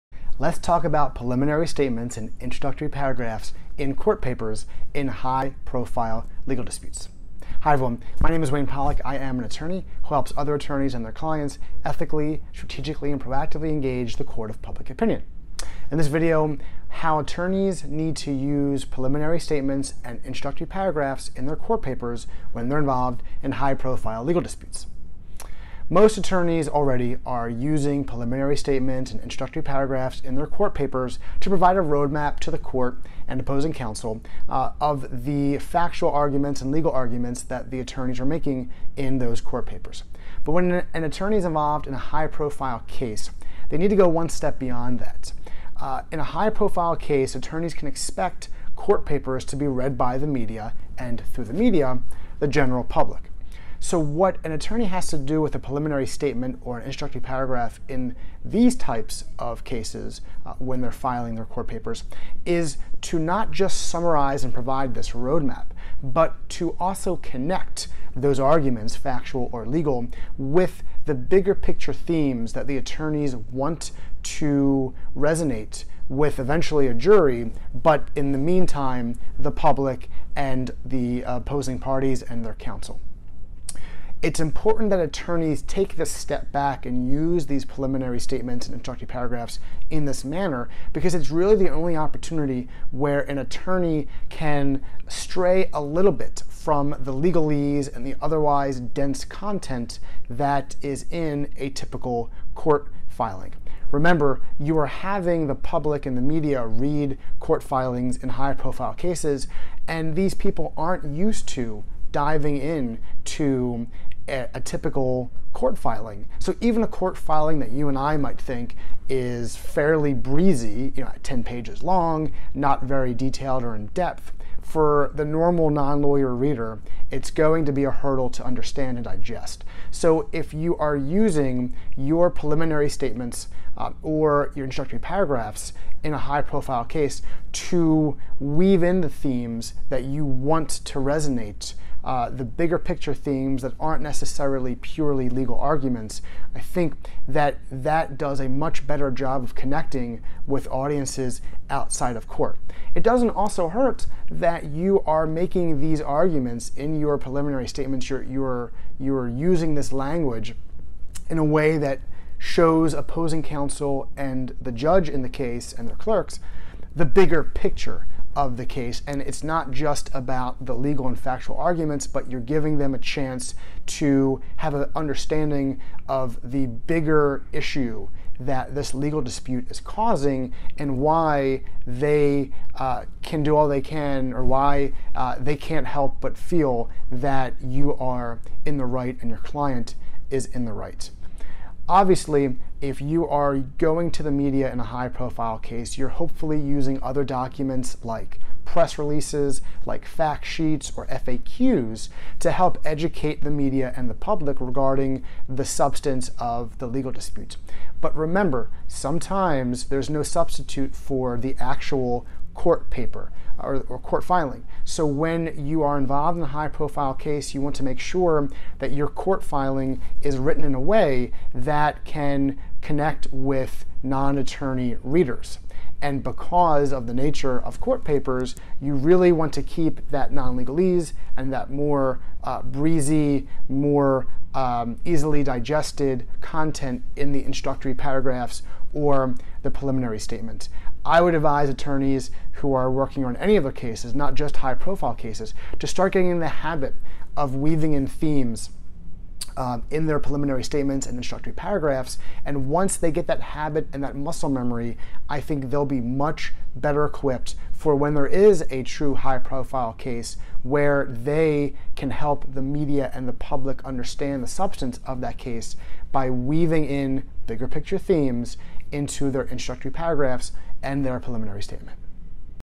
Use preliminary statements in your court papers to talk to the media and the public
In this video, Wayne Pollock (Founder and Managing Attorney of Copo Strategies) discusses how attorneys should be using the preliminary statements in their court papers when they are involved in high-profile, or potentially high-profile, legal disputes.

+ Download our "What to Say When a Reporter Calls" infographic